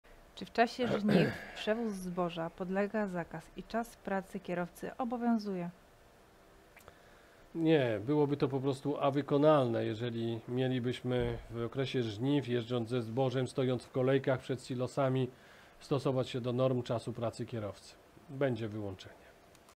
Czy w czasie żniw przewóz zboża podlega zakaz i czy czas pracy kierowcy obowiązuje?
Pytanie od kierowców zawodowych

00:00 - Czy w czasie żniw przewóz zboża podlega zakaz i czy czas pracy kierowcy obowiązuje?
00:07 - Przewóz zboża jest wyłączony z zakazów. Co do wyłączenia z czasu pracy, to przepisy tego nie regulują, czyli należy się do nich stosować.

System kierowca.pl kompleksowa obsługa szkoleń kierowców zawodowych.